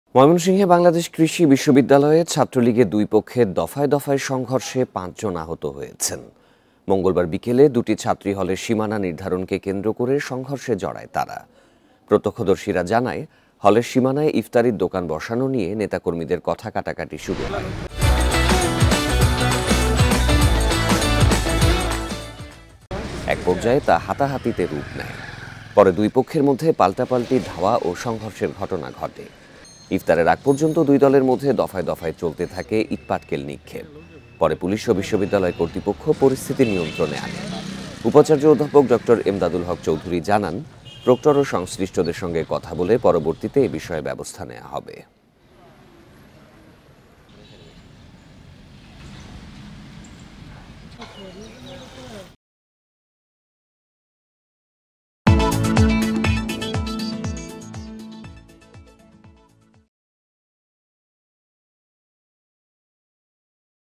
ছাত্রলীগের দুই পক্ষের দফায় দফায় সং ঘ র্ষ, আ হ ত ৫ | Mymensingh News | Independent TV
ময়মনসিংহে বাংলাদেশ কৃষি বিশ্ববিদ্যালয়ে ছাত্রলীগের দুই পক্ষের দফায় দফায় সং ঘ র্ষে ৫ জন আ হ ত হয়েছেন। মঙ্গলবার বিকেলে দুটি ছাত্রী হলের সীমানা নির্ধারণকে কেন্দ্র করে সং ঘ র্ষে জড়ায় তারা